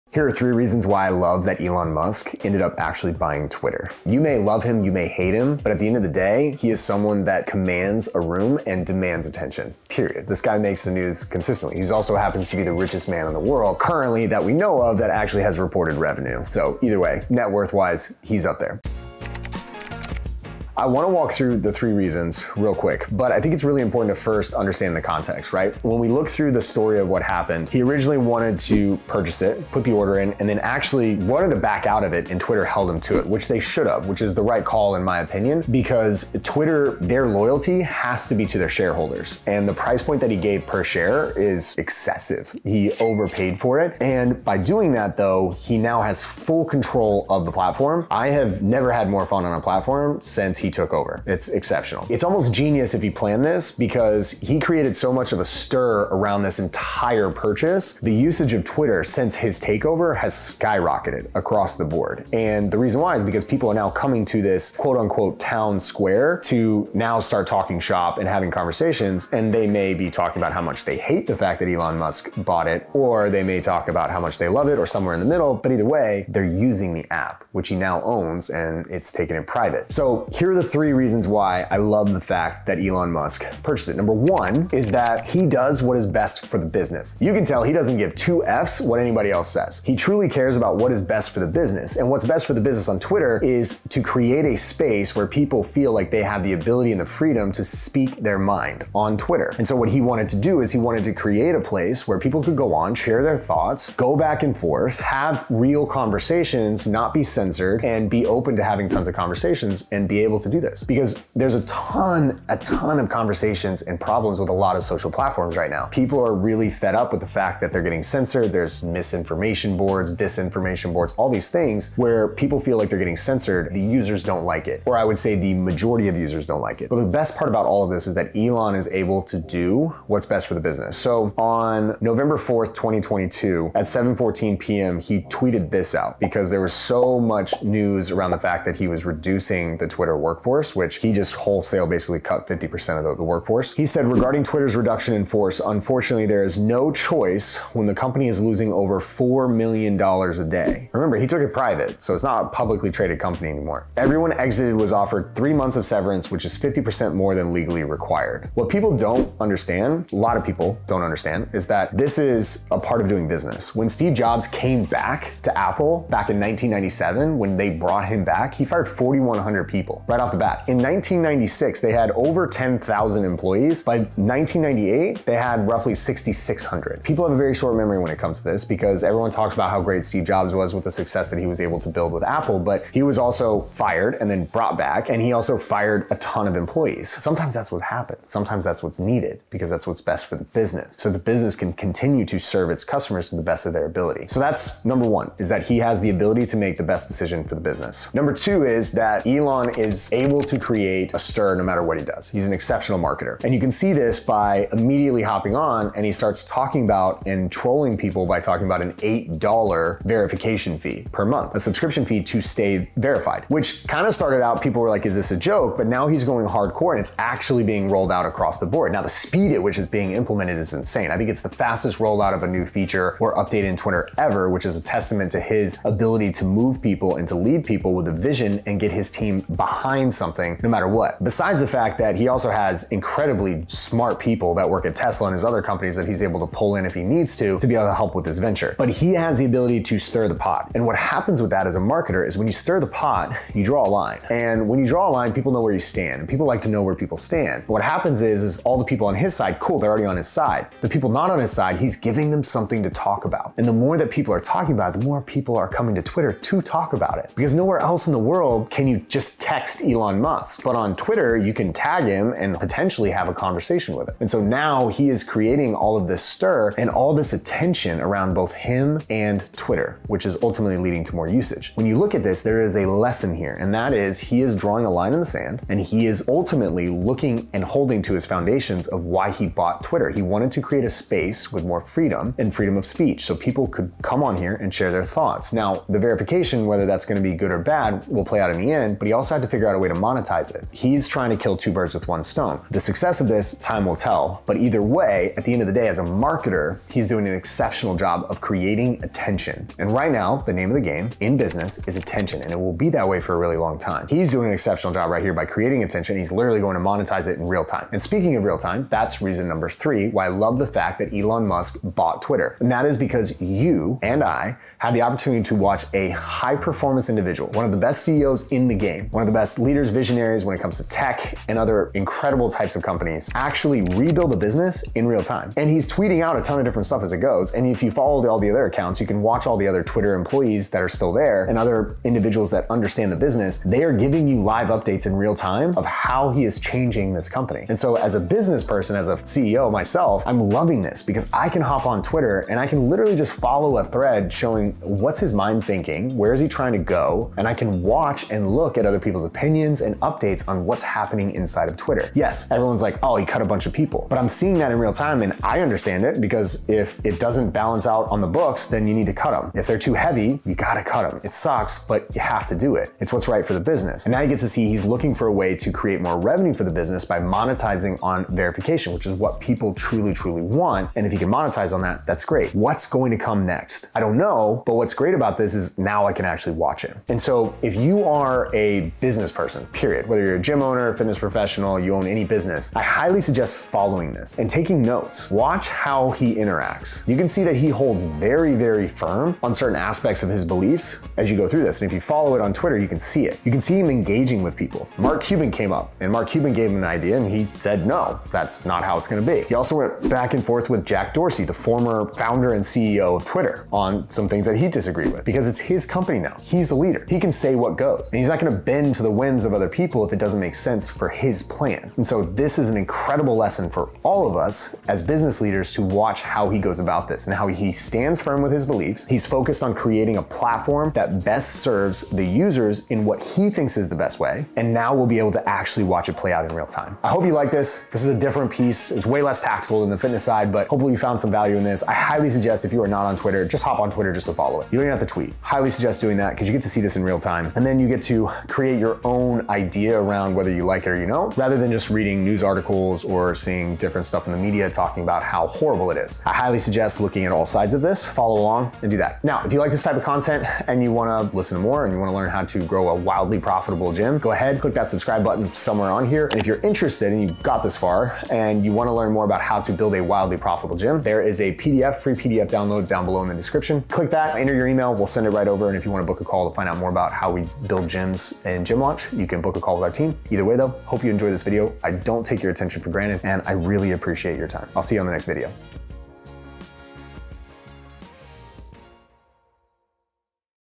3 REASONS Why ELON MUSK Buying TWITTER Is An Amazing Thing.
There's been quite a stir recently about Elon Musk buying Twitter. Everybody seems to be talking about it, some think it's a good thing and some think it's a complete disaster.

This is our take on the situation. There's definitely some BIG LESSONS to be learned from all of this if you're a business owner yourself. Enjoy the video and let us know what your thoughts are on this situation in the comments below.

Want to build a $1M Gym and listen to great speakers like Alex Hormozi and Alex Rodriguez in a LIVE setting?

Wanna Run Profitable Fitness Ads & Get More Members In Your Gym?